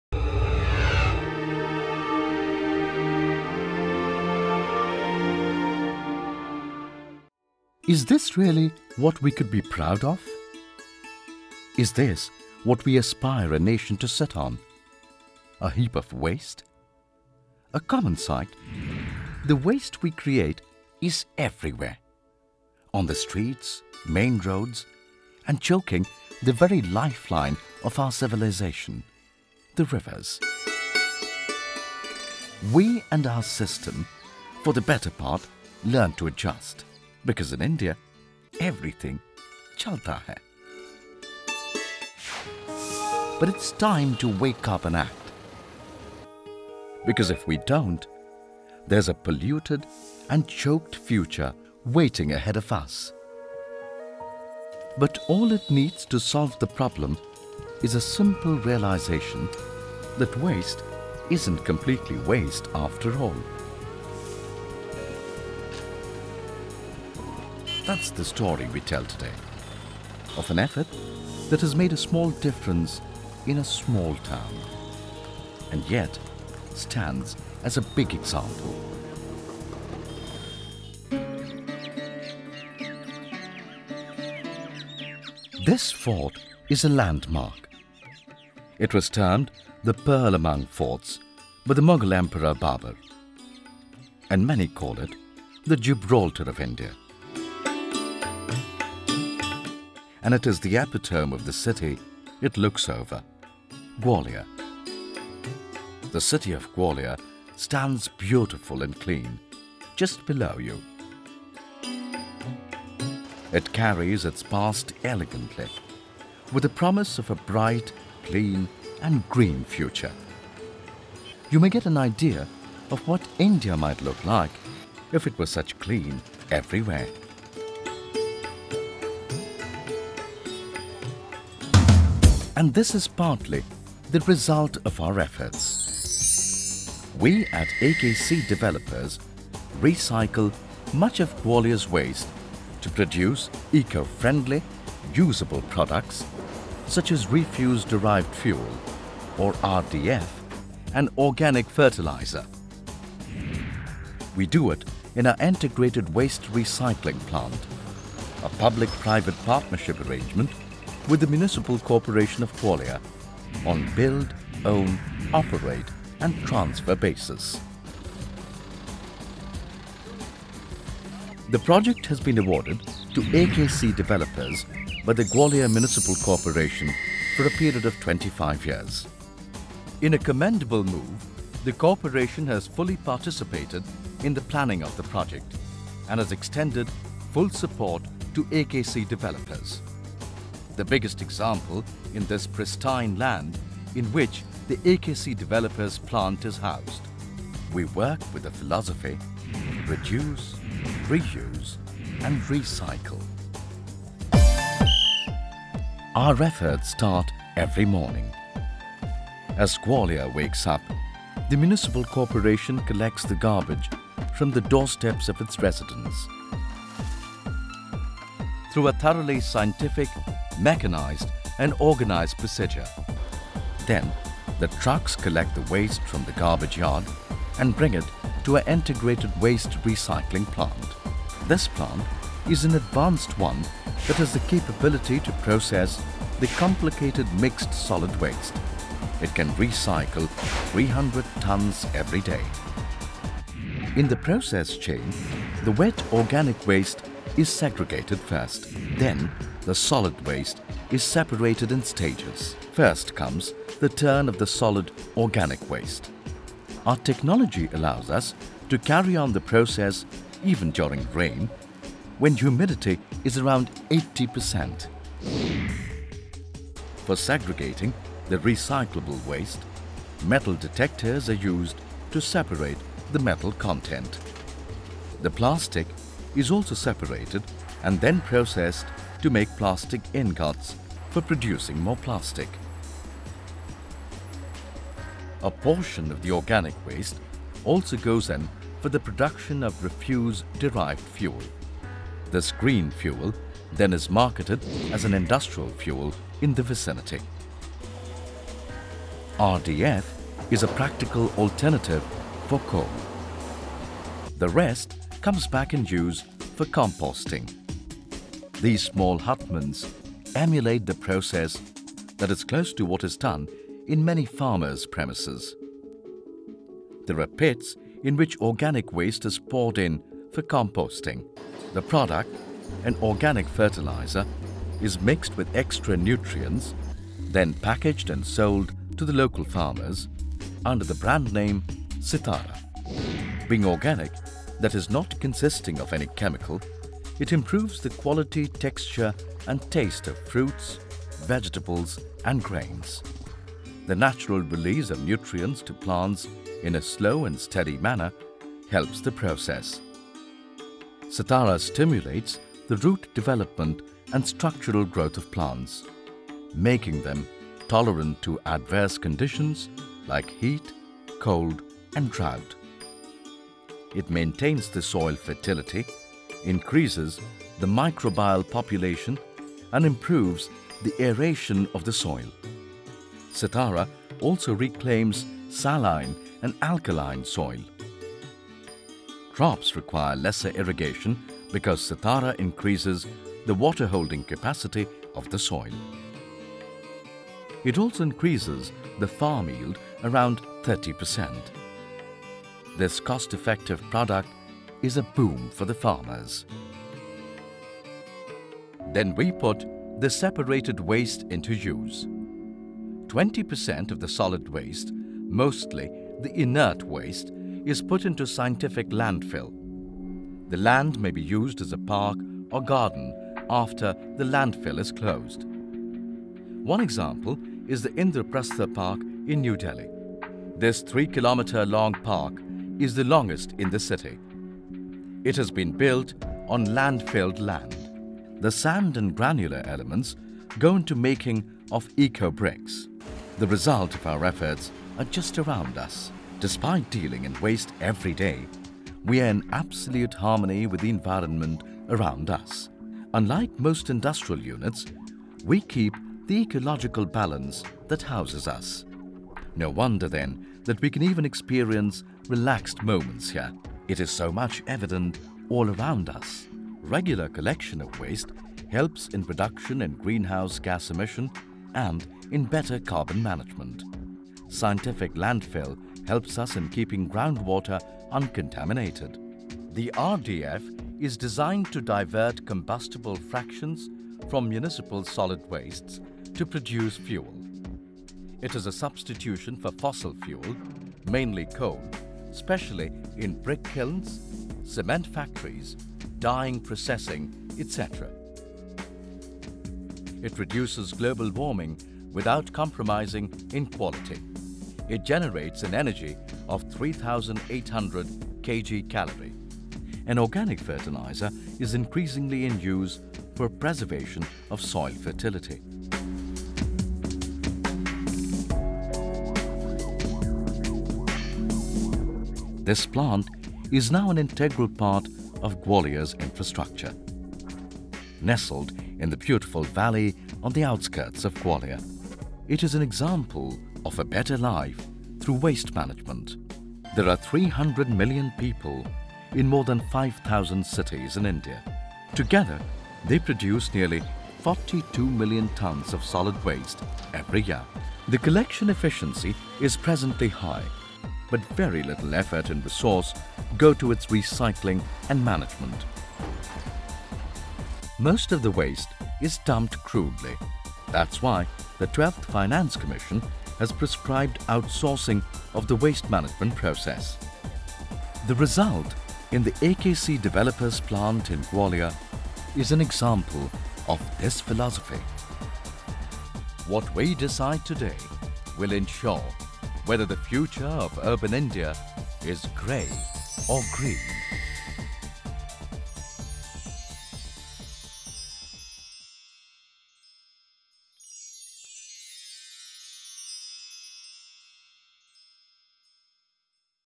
Don't Waste The Waste
This film was made in the month of September,2009 the idea was to show the waste management process by AKC Developer limited in their top of the line plant at Gwalior city. The project is a BOOT project and has been awarded to ACK Developer for 25 years. The project is an excellent example of the waste management wherein Refused Derived Fuel - Composed Fertilized and many more products are being produced. All these are being shown in the film, it was a experience by itself to produce the film within given time.